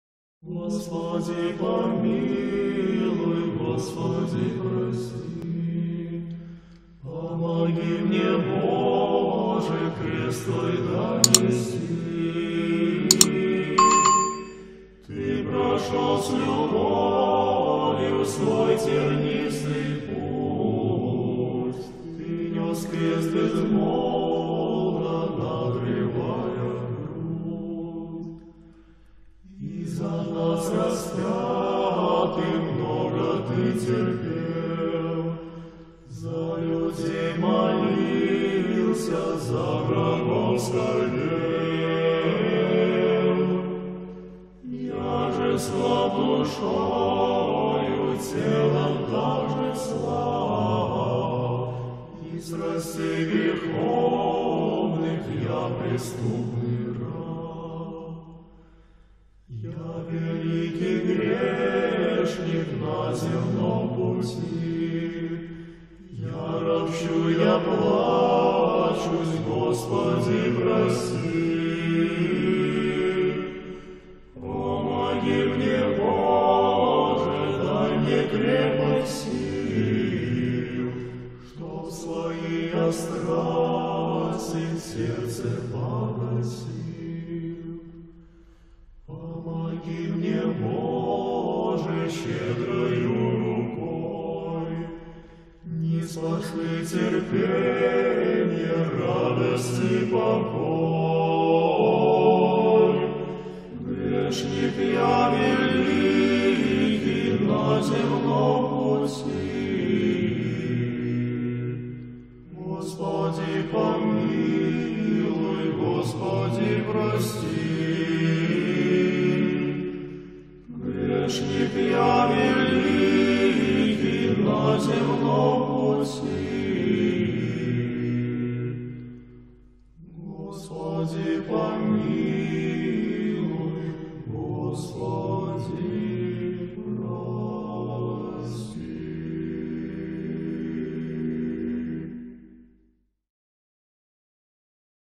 [T-VLOG](치 타) 성 바스크리센스키 교회(러시아 여행)
성 바스크리센스키 교회는 1936년에 치타의 모든 교회가 철폐되고, 파괴된 후,1994년 재건축 된 이후부터 치타의 러시아 정교회의 부활의 상징인 된 교회입니다.